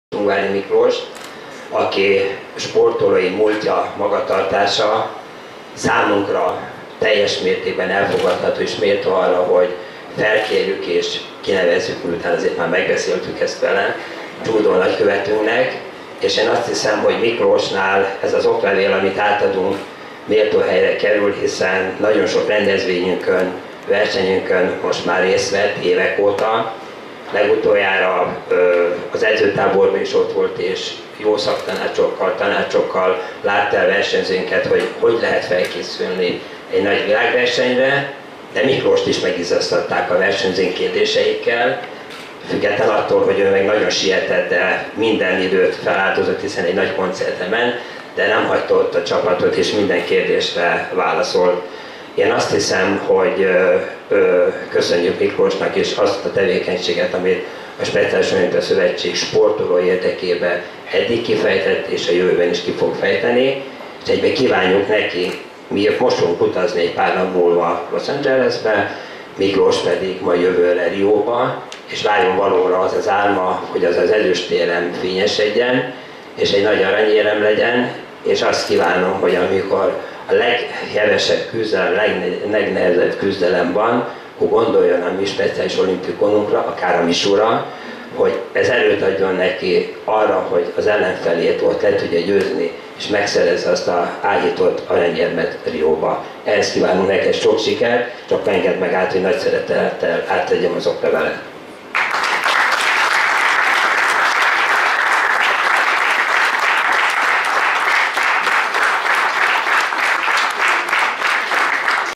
Ungvári Miklós lett az MSOSZ Judo-nagykövete
Az olimpiai ezüstérmes cselgáncsozó, Ungvári Miklós lett a Magyar Speciális Olimpia Szövetség judo-nagykövete. A bejelentés után Lengyel Lajos elnök egy oklevéllel ajándékozta meg a sportolót.